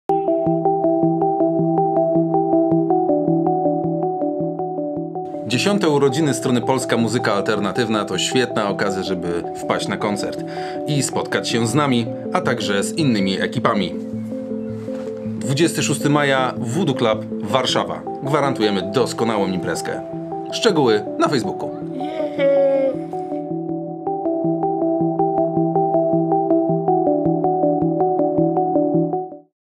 Infinitone zaprasza na koncert 26 maja VooDoo Club w Warszawie.
Razem z zespołem Infinitone z Poznania zapraszamy Was na naszą urodzinową bibę organizowaną wspólnie z zespołami Infinitone, Tattva, Grupa Ż i Druides, które zagrają specjalnie dla Was! Będziemy bawić się w klimatach rockowo-metalowych a w barze piwko, drinki i inne takie 🍺🍷🍹

🆁🅾🆉🅺 Ł🅰🅳⠀⠀🅹🅰🆉🅳🆈:
MIEJSCE: 𝐕𝐨𝐨𝐃𝐨𝐨 𝐂𝐋𝐔𝐁 𝗪𝗮𝗿𝘀𝘇𝗮𝘄𝗮, al. Prymasa Tysiąclecia 48A
DATA: 𝗰𝘇𝘄𝗮𝗿𝘁𝗲𝗸, 𝟮𝟲 𝗠𝗔𝗝𝗔 𝟮𝟬𝟮𝟮 𝗿.
OTWARCIE BRAMEK: 𝟭𝟴:𝟬𝟬
STARTUJEMY: 𝟭𝟵:𝟬𝟬

Zapraszamy :)